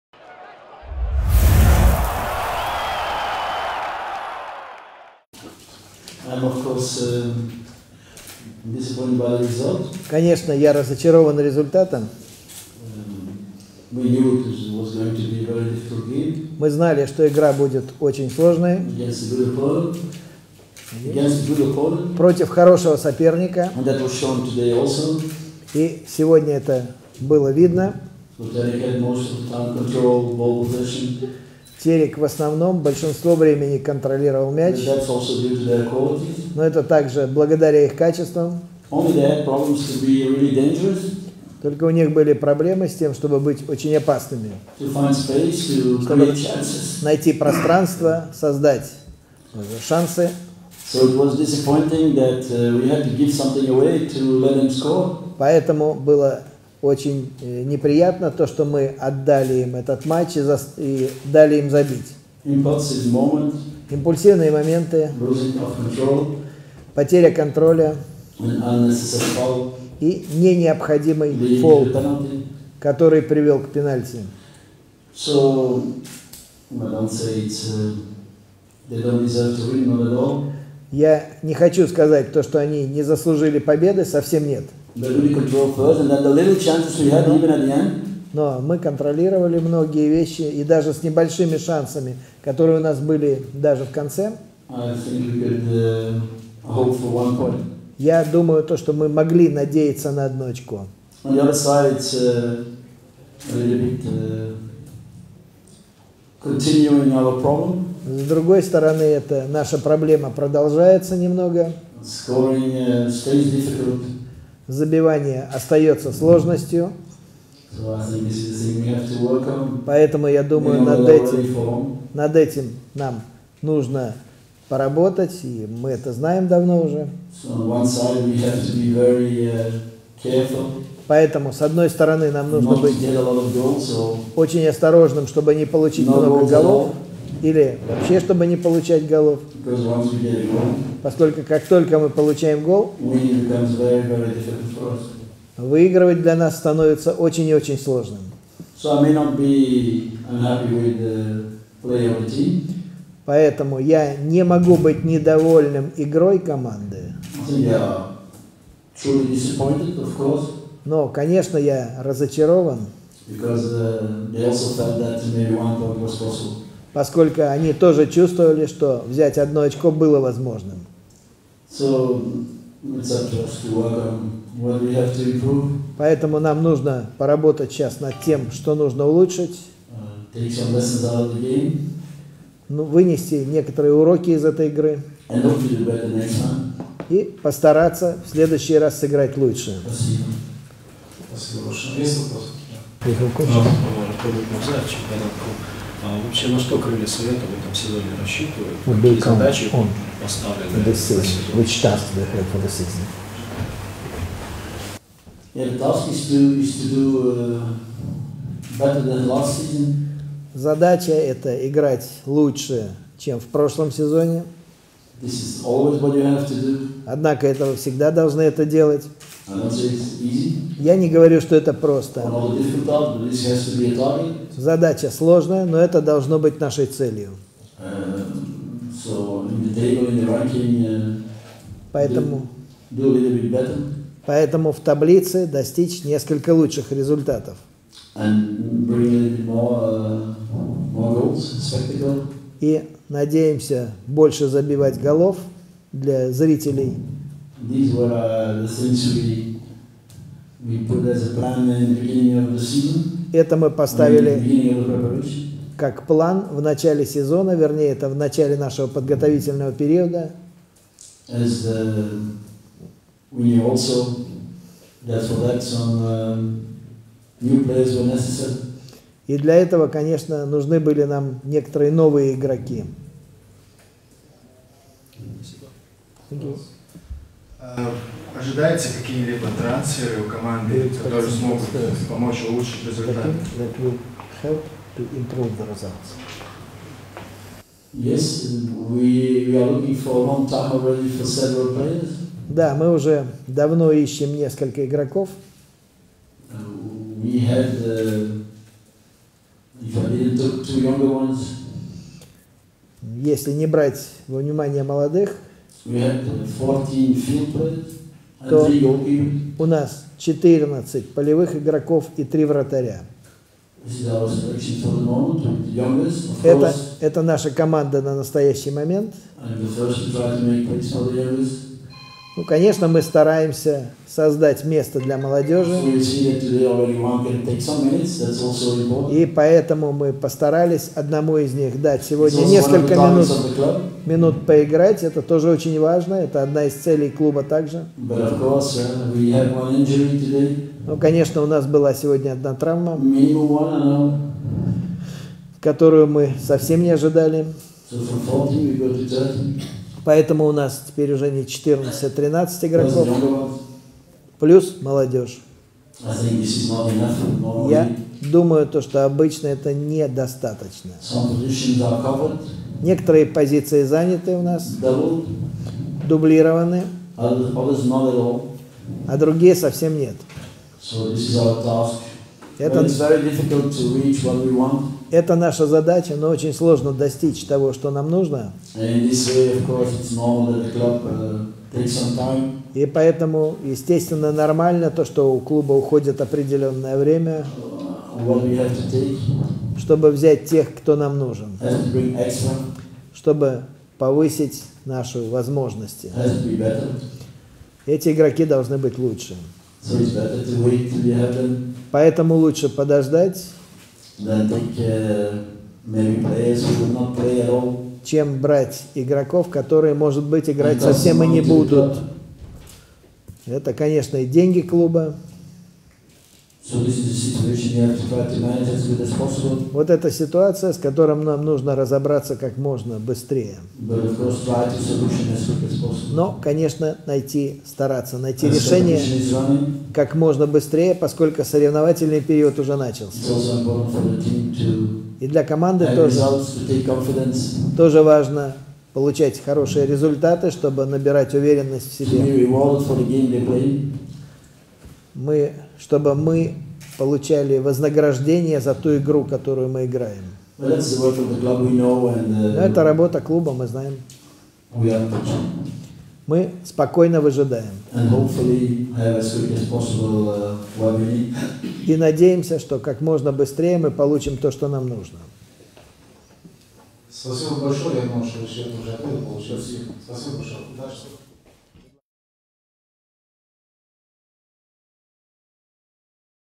Франк Веркаутерен: Мы разочарованы тем, что проиграли этот матч - "КС-ТВ"
Пресс-конференция главного тренера "Крыльев Советов" Франка Веркаутерена после матча с "Тереком".